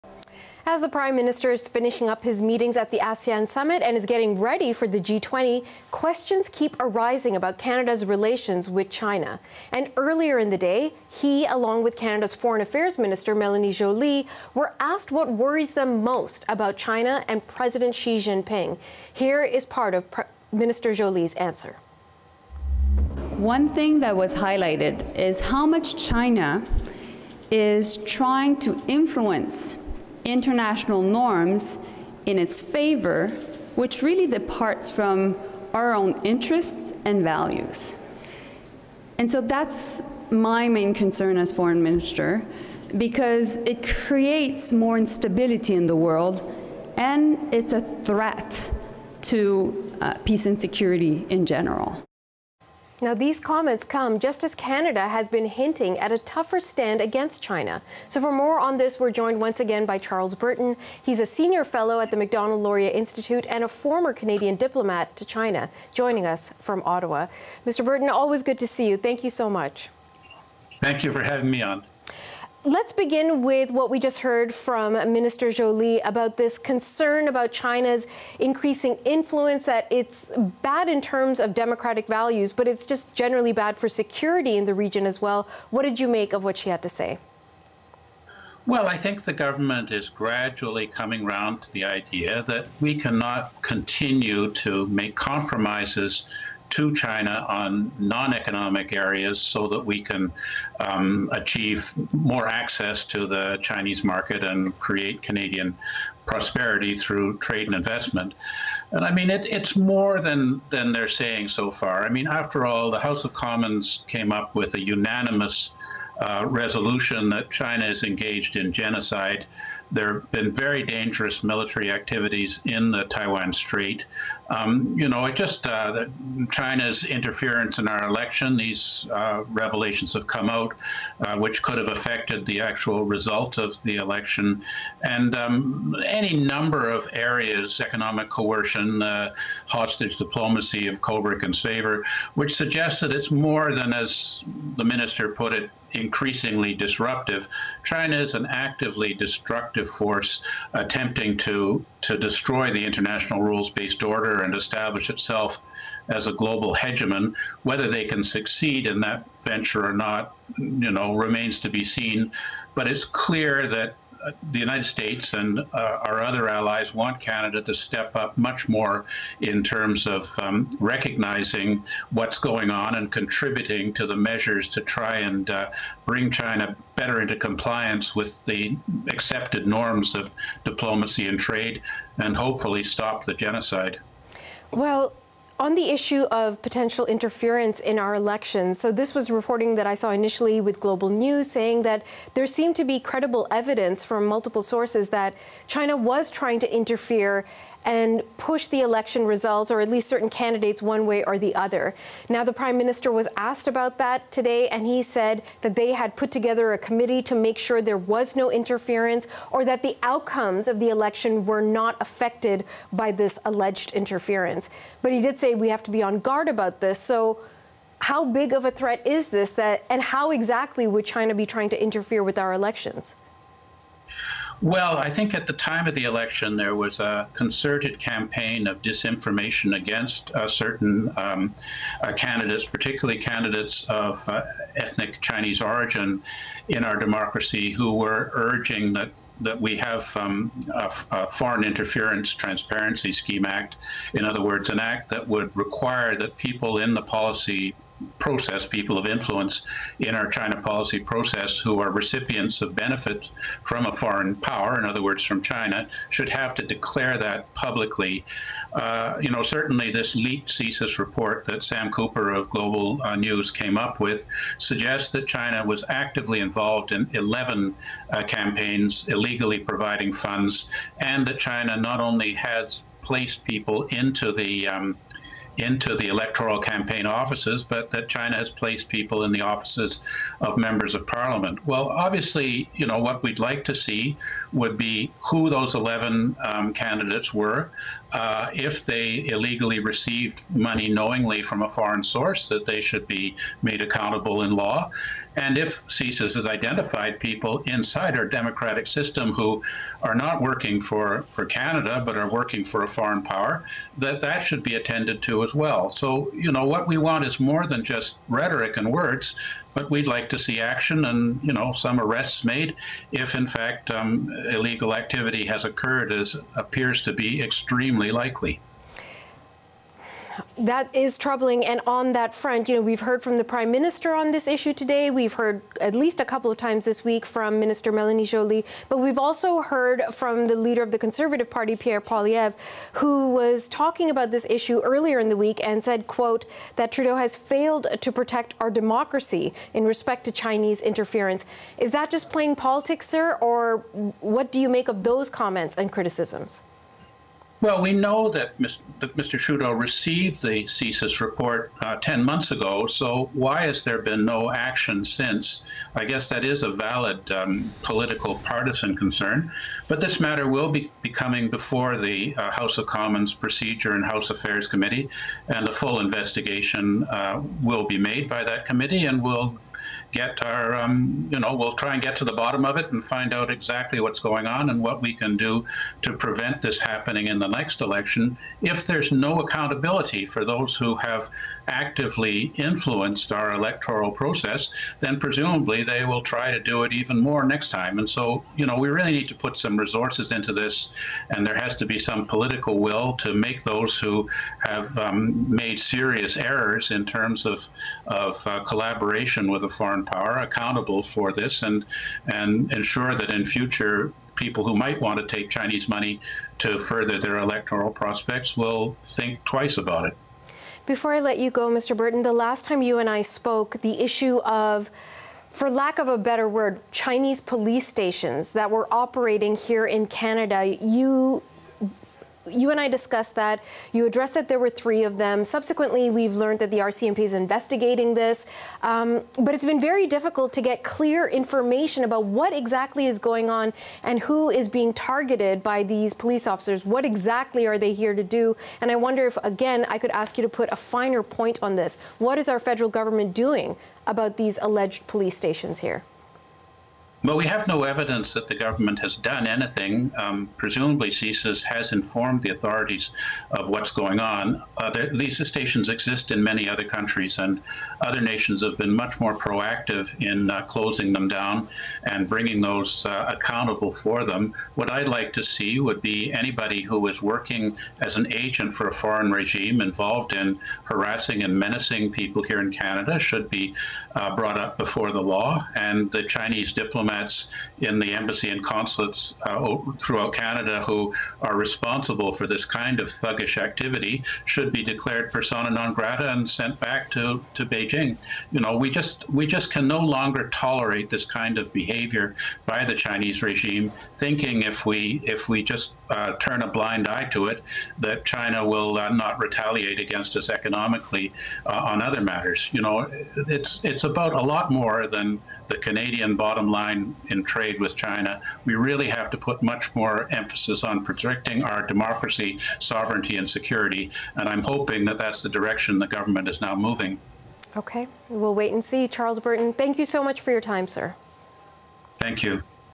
Reports China tried to influence federal election warrant further action, says former diplomat
Former Canadian diplomat to China Charles Burton discusses the federal government's approach to China and Prime Minister Justin Trudeau's response to CSIS documents reportedly showing efforts by China to influence Canada's 2021 federal election.